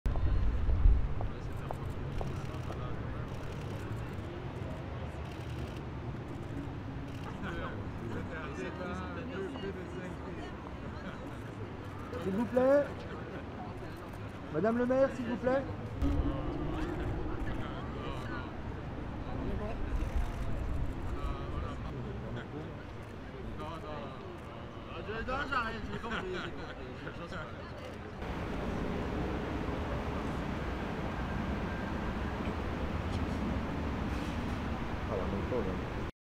JO-2024: Thomas Bach devant les anneaux olympiques à Paris
Le président du CIO Thomas Bach est arrivé dimanche à Paris, où il a été accueilli par la maire Anne Hidalgo et le futur président du comité d'organisation des JO-2024 Tony Estanguet devant les anneaux olympiques installés place de l'Hôtel de Ville. Durée: 00:36